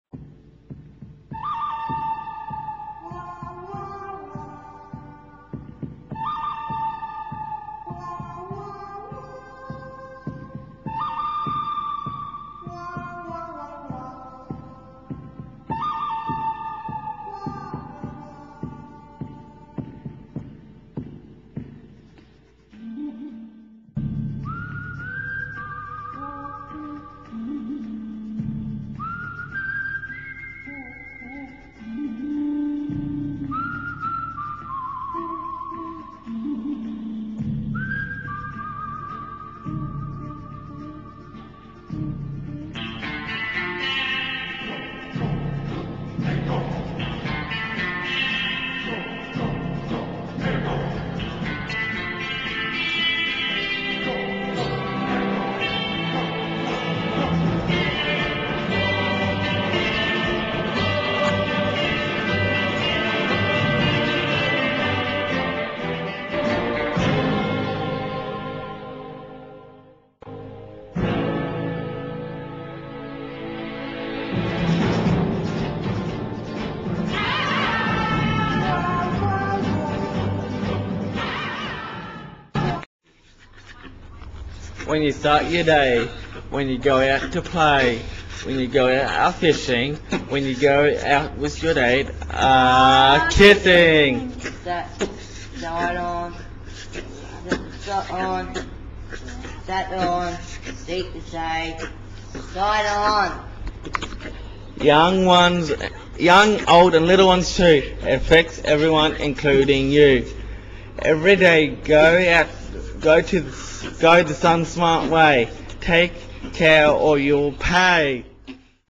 Sunsmart showdown.wmv
This entertaining video was made by a group of QLD teens for Cancer Council Australia's SunSmart competition. It's in the style of a Sergio Leone Spaghetti western 'showdown' and was a runner-up in the movie category of the comp. It was made by Paul Rahme, Kerrod Hamilton and Michael Y.